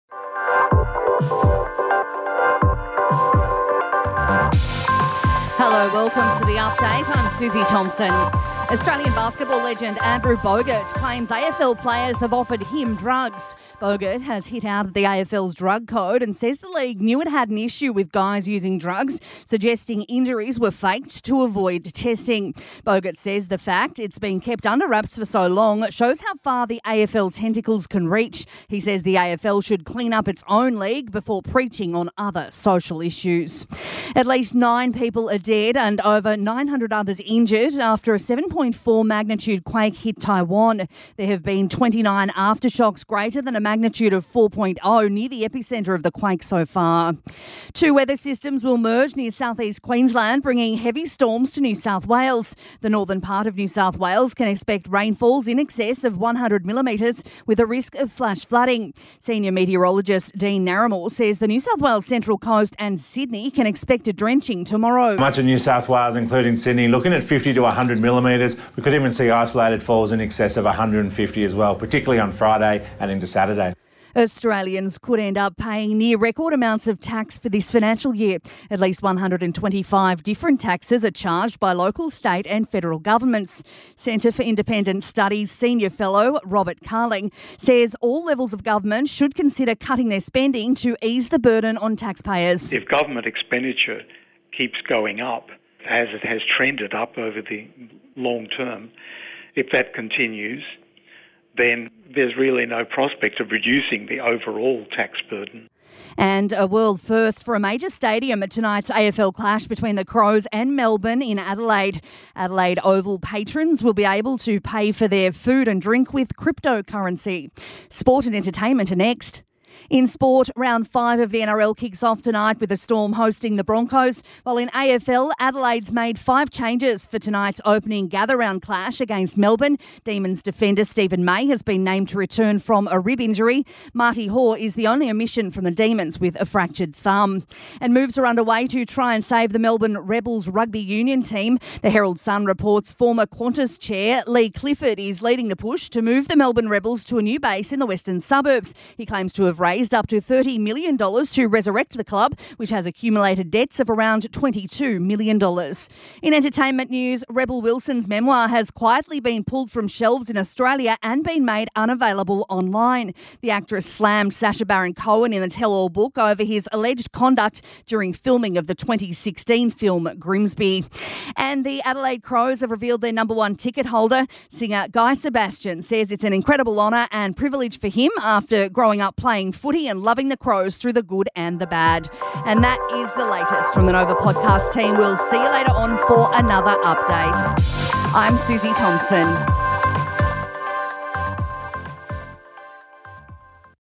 Basketball Star Slam Dunks AFL Further into Drugs Drama
Andrew Bogut claims he's been offered drugs from AFL players

The death toll rises after Taiwan quake

Adelaide Oval to be the first major stadium in the world, to accept cryptocurrency

And why Rebel Wilson's memoir is being quietly removed from shelves across Australia